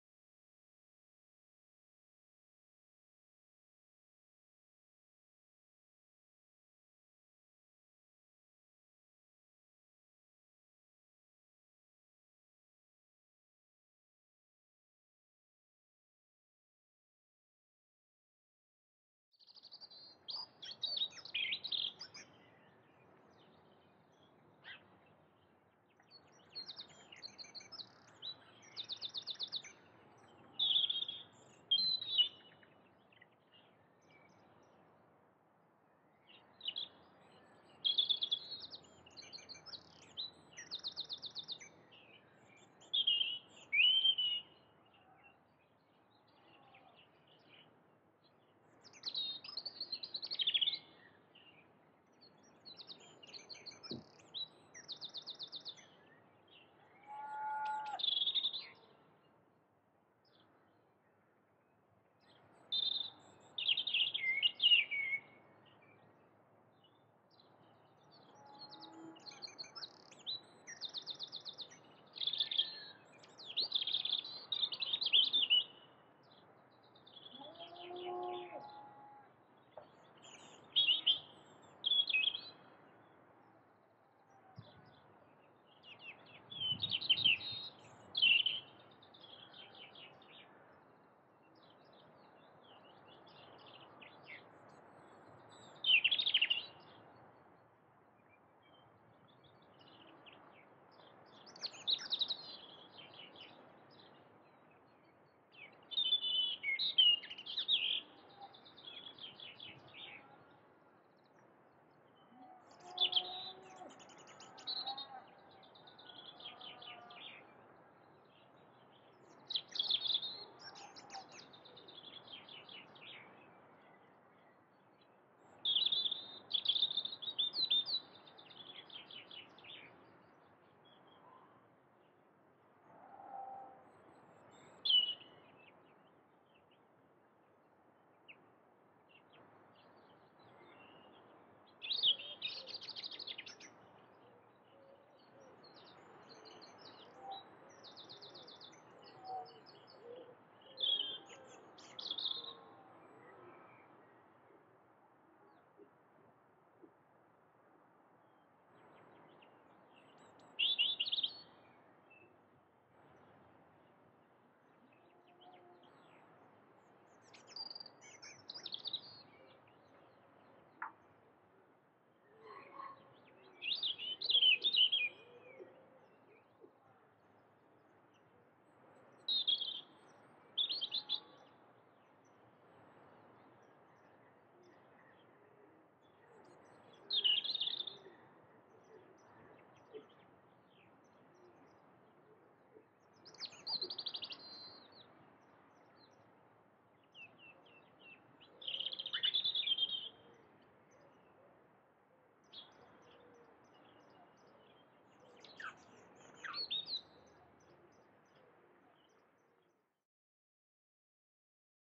Relaxing Cat Video Series - Daydreams - E01 (beautiful, calming scenes of cute silver tabby kitten)
Soothing cat video with beautiful pictures and calming bird song for intense daytime relaxation, stress or anxiety reduction. Also check out the season 'Goodnight' for sleep aid or nighttime relaxation. :)